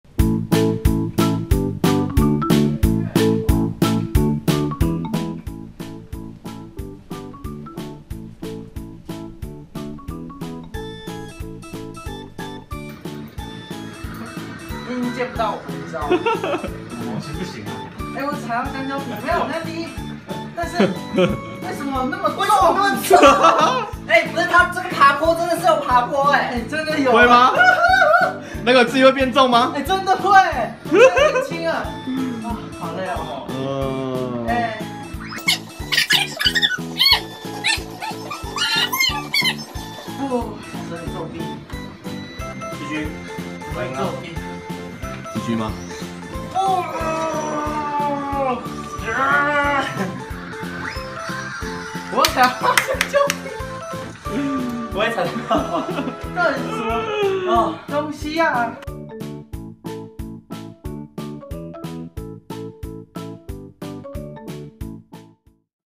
閃電狼 FW x LoL｜電狼日常：健身車比拼｜Exercise Bike race：Betty vs SwordArT!!
某天團練結束的晚上，在休息室裡面，有兩名選手默默的在進行他們的對決...